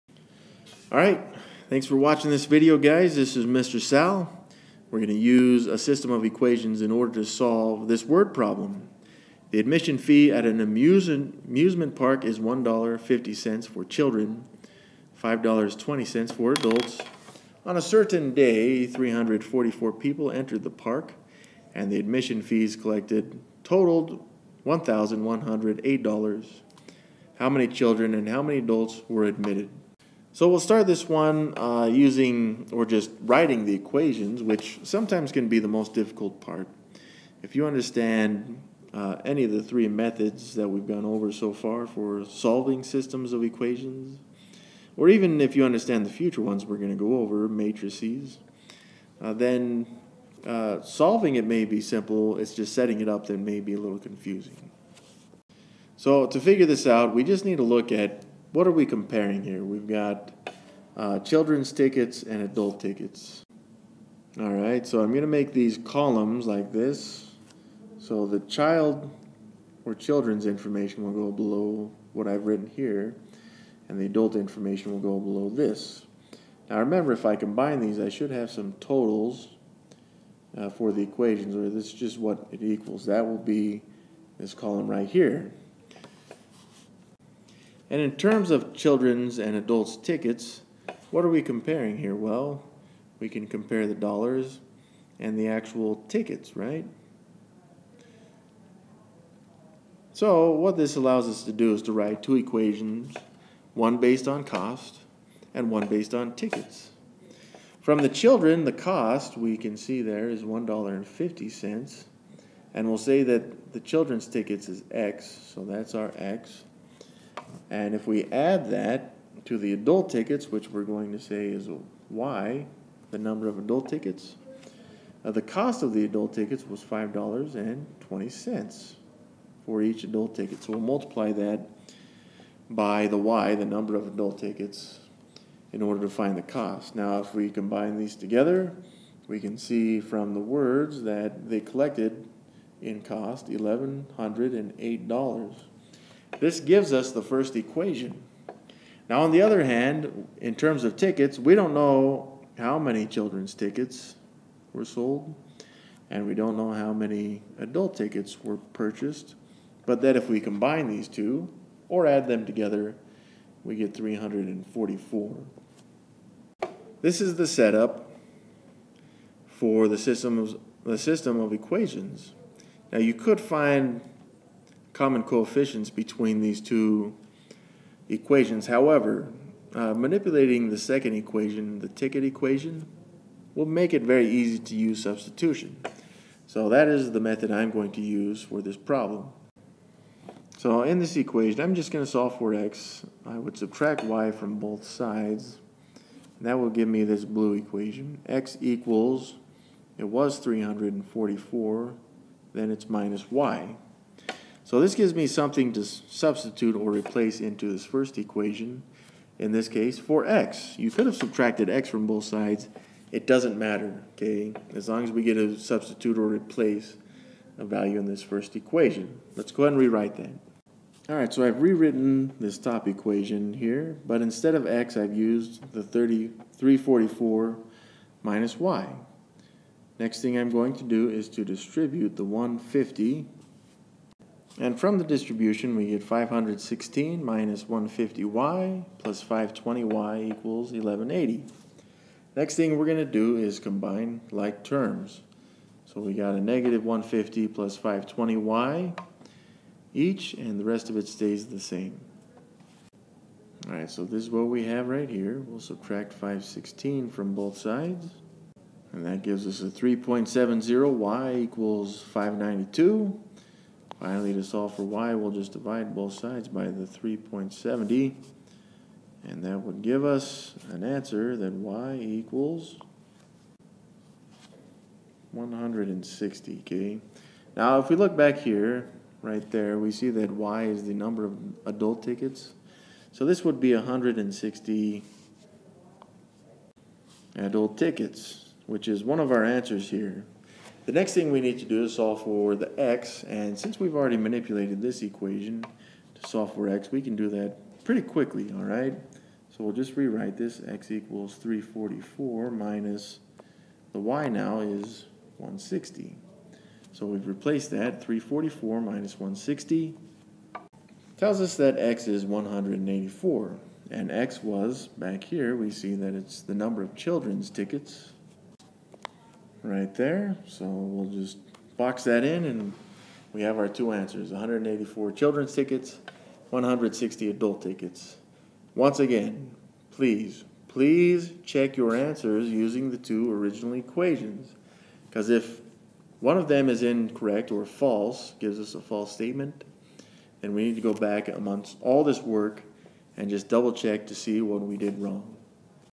Write and Solve a System of Equations: Purchasing Children's and Adult Tickets
The problem: The admission fee at an amusement park is $1.50 for children and $5.20 for adults. On a certain day, 344 people entered the park, and the admission fees collected totaled $1108. How many children and how many adults were admitted?

The solution: 184 children's tickets and 160 adult tickets